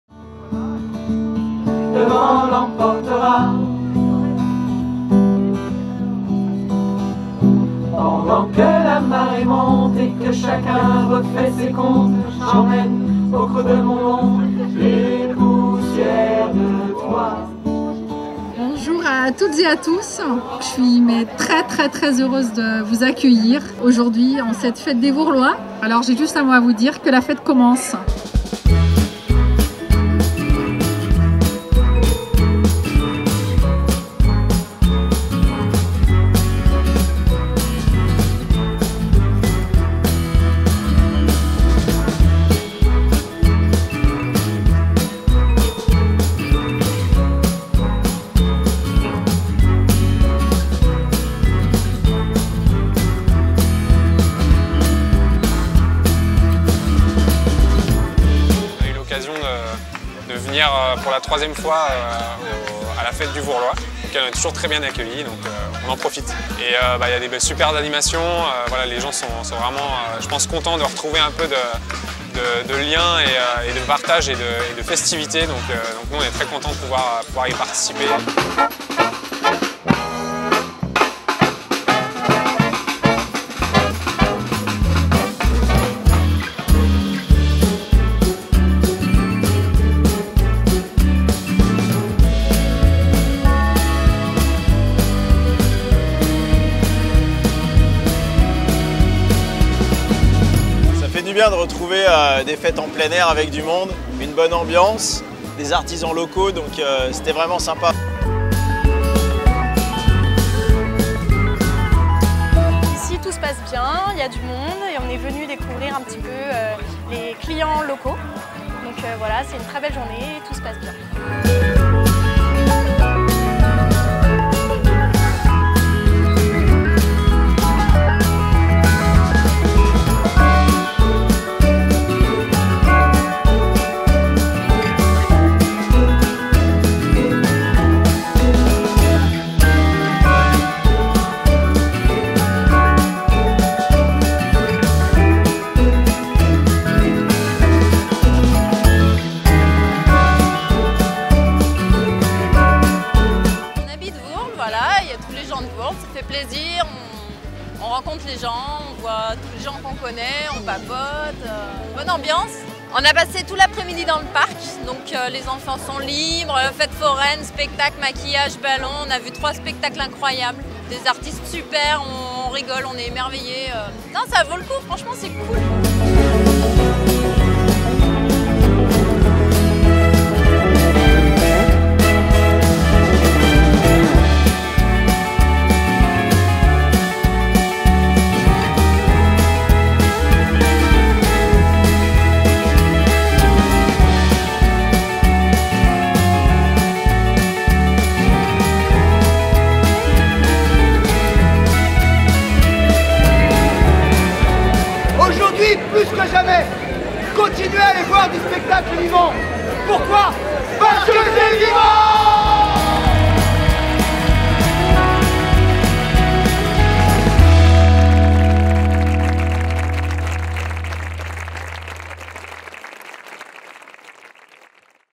Fête des Vourlois 2021
Retour en image sur la Fête des Vourlois du dimanche 10 octobre 2021.
Des spectacles, des produits artisanaux, des animations et surtout de la bonne humeur !